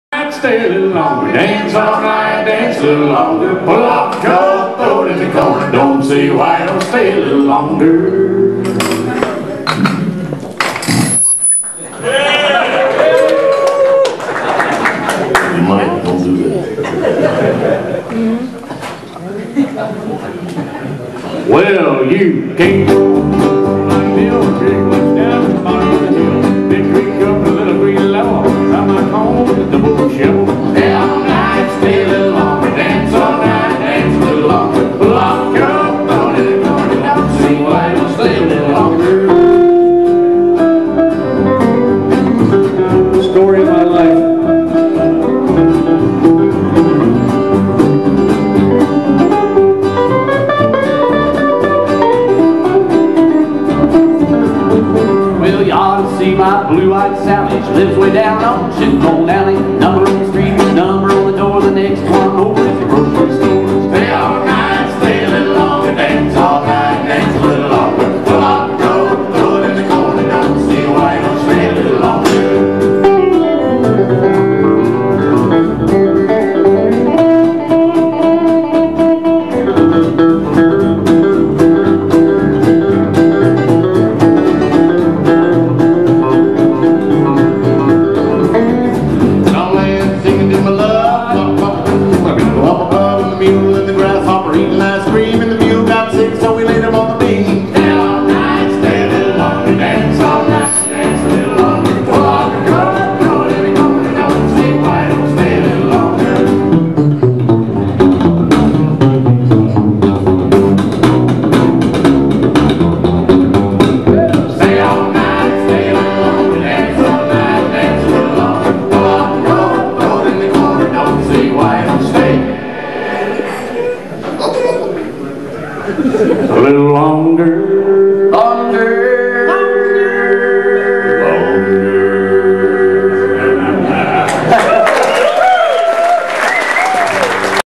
Dale Watson & Friends
Dale Watson & Chuck Mead
special guests: Redd Volkaert (gitar) and Mark Miller (bas)
Live in Het Oude Theater, Oss 2009